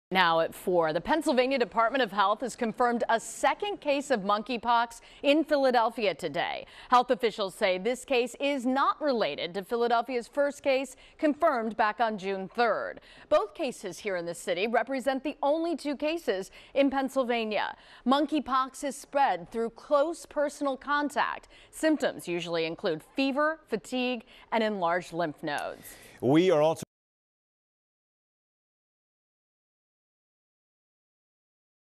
Health officials report 2nd probable case of monkeypox in Philly; say it is not related to 1st case
The Pennsylvania Department of Health reported a second probable case of monkeypox in a Philadelphia resident on Wednesday.

Health officials said the case is not related to Philadelphia's first case, which was confirmed on June 3.